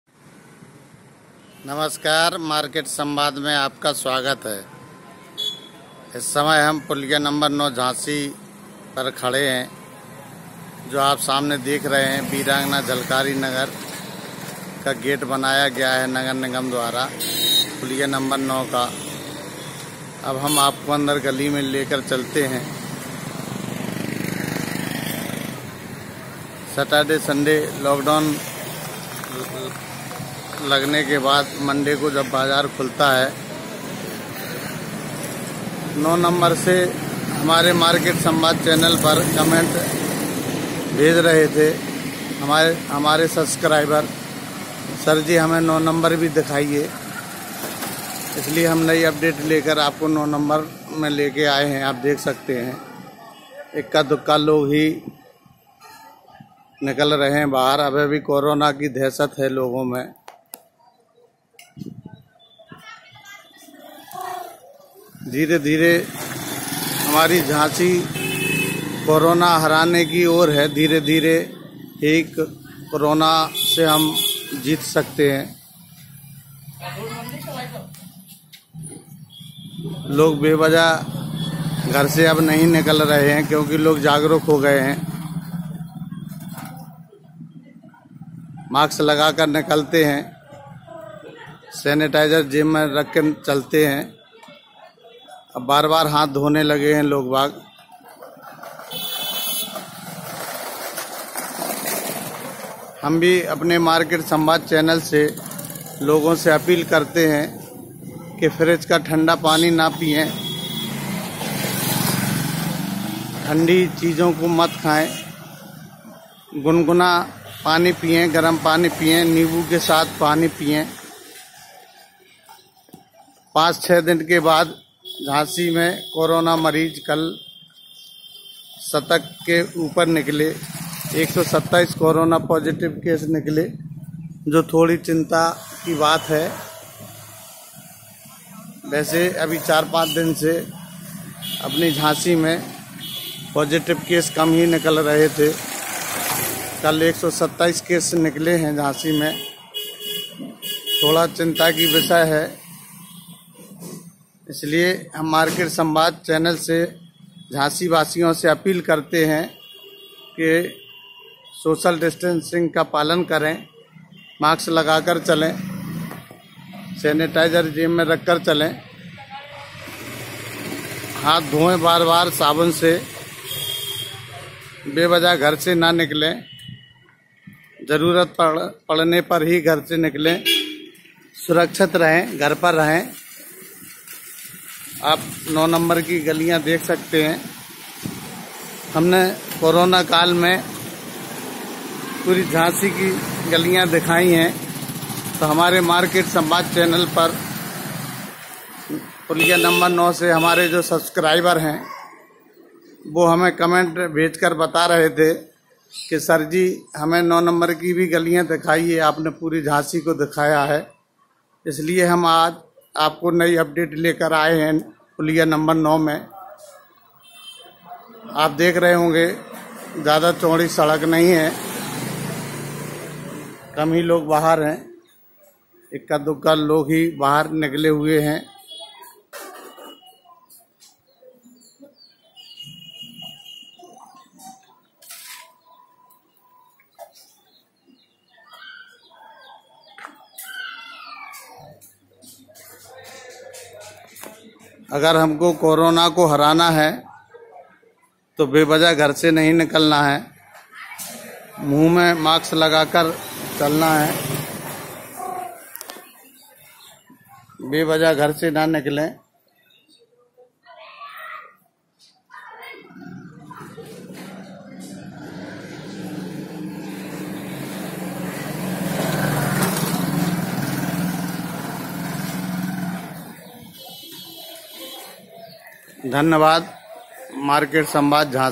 झाँसी-वीरांगना झलकारी नगर, पुलिया नम्बर -9, रिपोर्ट-अनिल मौर्य
झांसी के प्रेम नगर थाना क्षेत्र का पुलिया नंबर 9 इलाका काफी प्राचीन इलाका है यहां पर मिश्रित आबादी रहती है। कोरोना काल में गलियों में सन्नाटे की स्थिति है लोग घरों से नहीं निकलते हैं क्योंकि वह गाइडलाइन का पालन कर रहे हैं । यहाँ क्या हालात है इसको हम वीडियो के जरिए आपको दिखा रहे हैं